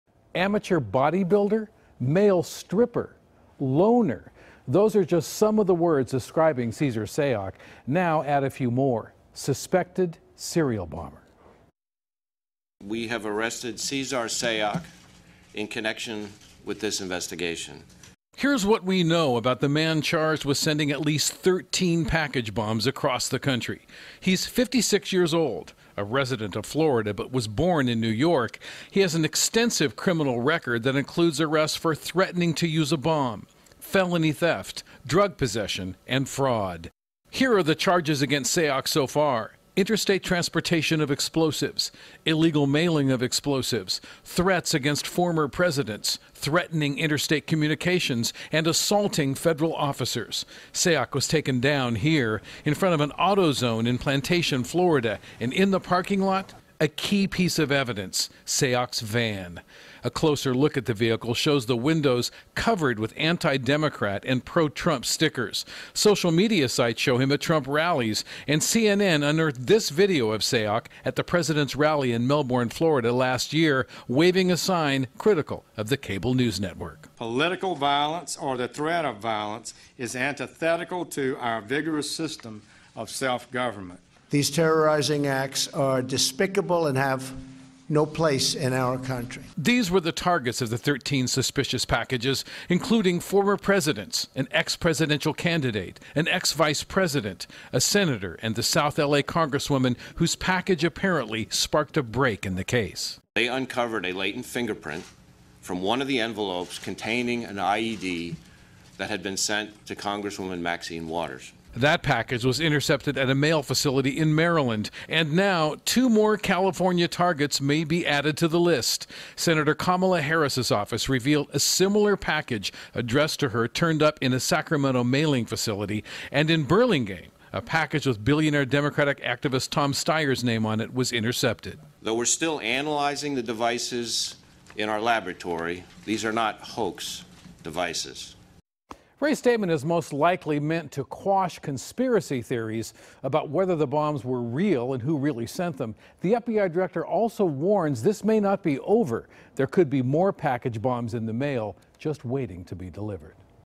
Package Bomb Suspect Arrested, Cesar Sayoc Charged With 5 Federal Crimes
Following a nationwide manhunt, federal authorities have arrested 56-year-old Cesar Sayoc, a Florida man accused of sending a series of explosive devices to prominent Democrats and critics of President Trump. Randy Paige reports. Check here for live updates